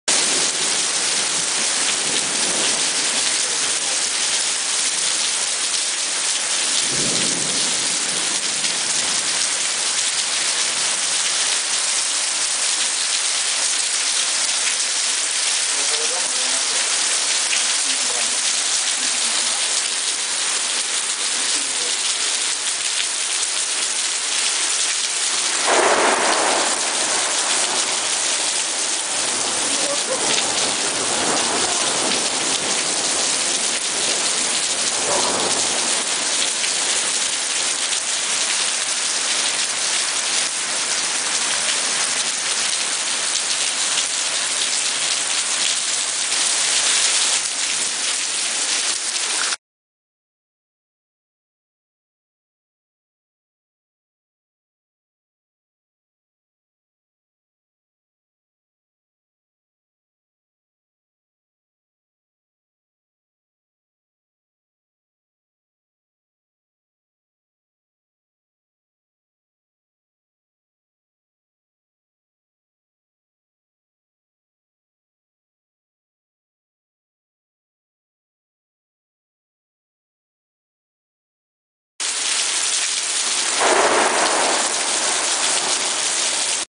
Застигнутые дождем
Россия. Москва Воробьевы горы. Нескучный сад. Дождь. Гроза. Moscow, Russia. Sparrow Hills. Neskuchny Garden. Июнь 2012г.